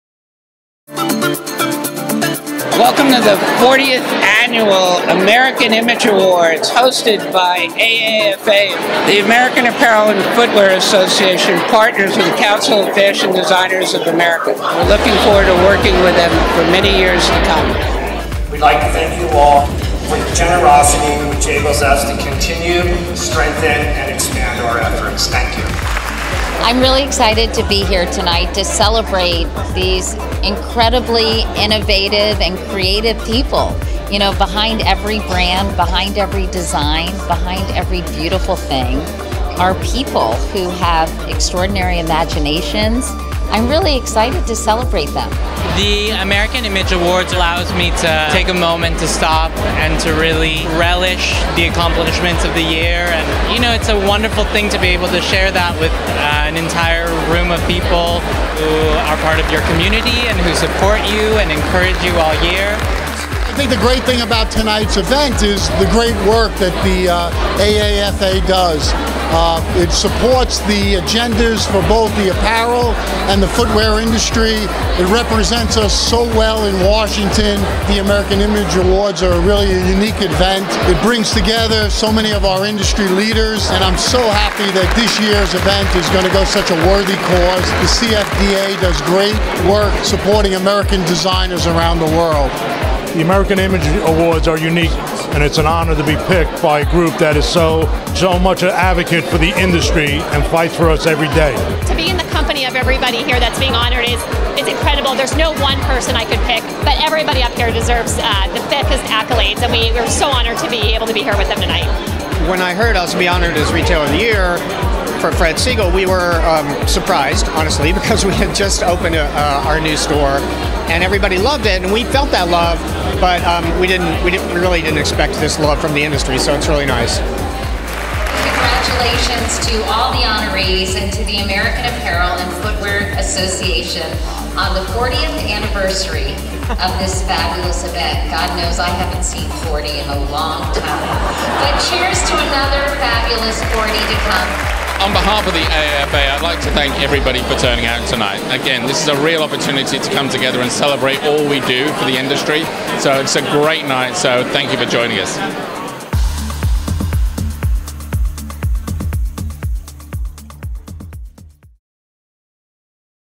40th AAFA American Image Awards
On April 16, 2018, AAFA hosted its 40th AAFA American Image Awards celebrating the accomplishments of the fashion and retail industry. Hosted by award-winning journalist and New York Times best-selling author Katie Couric, the gala honored leaders, influencers, and innovators in the apparel and footwear industries and benefitted the Council of Fashion Designers of America (CFDA) Foundation. Each year, the AAFA American Image Awards honors those who have exemplified leadership, excellence, and outstanding achievement in all sectors of the apparel and footwear industry, including design, manufacturing, and retail.